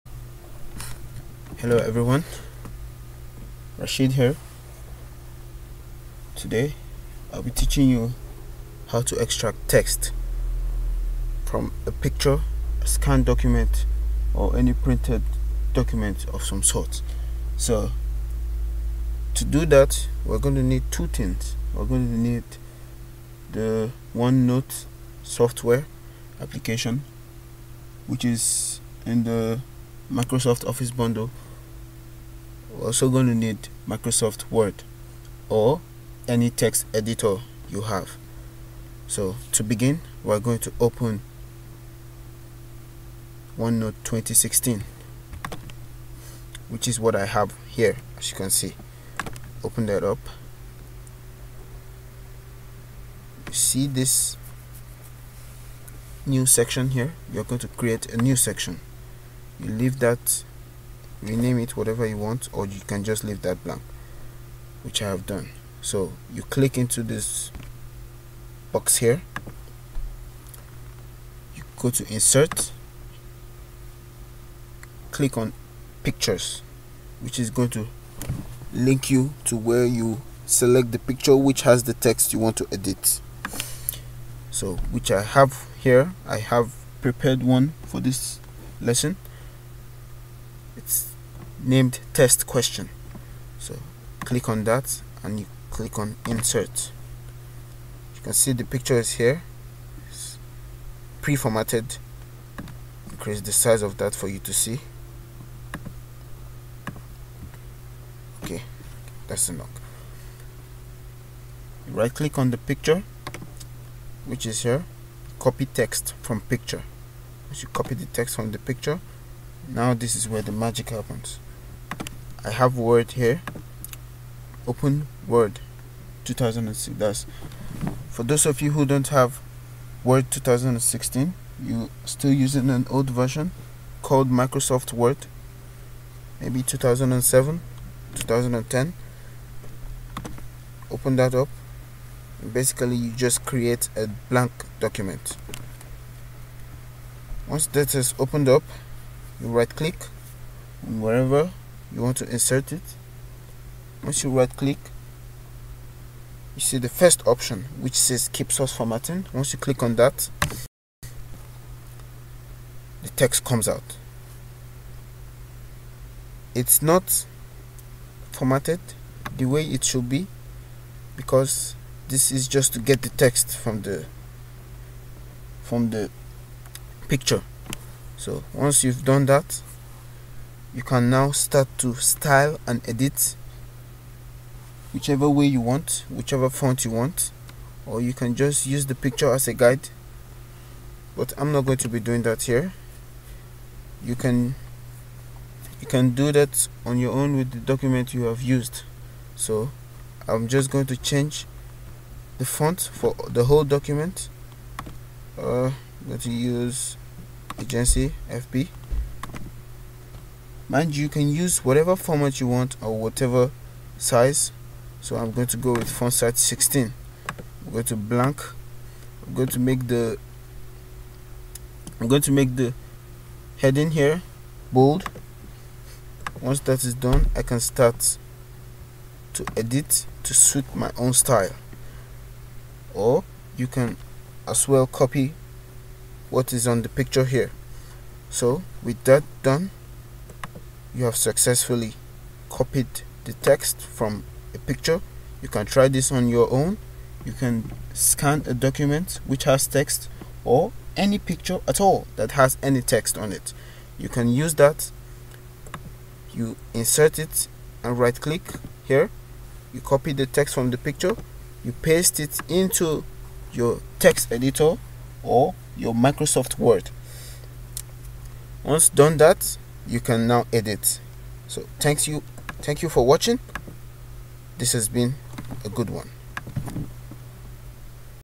How to Extract and Edit Text From a Picture or Scanned Document!!!
Hello again, today i'll be showing you how to extract text from a picture or a scanned document, you'll be needing 2 softwares: OneNote and Word(Microsoft Word), if you have it, then just follow me! if you don't understand anything here, lemme know in the comments!